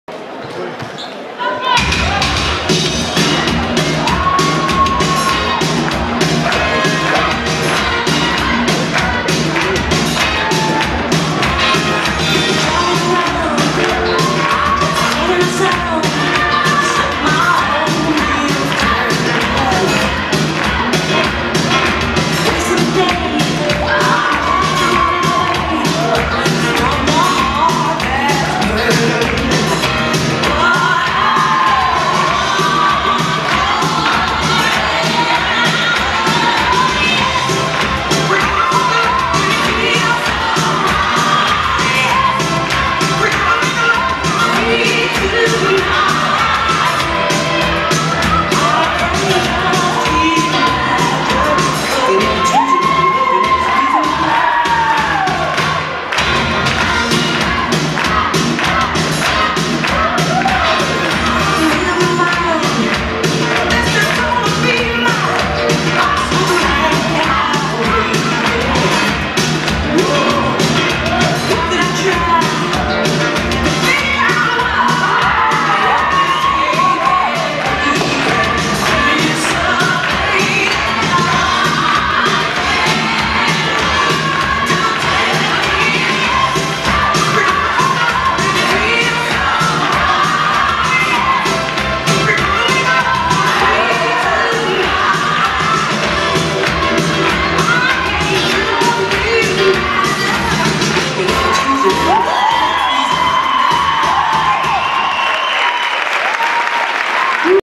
GPvD 2013 - Michelle Uhl & Kris-Marcel Fischer
Am 20. April 2013 haben wir den Großen Preis von Deutschland in Worms ausgerichtet. In der C-Klasse hat unser Paar Michelle Uhl & Kris-Marcel Fischer mit dieser Endrundendarbietung den 1. Platz errungen.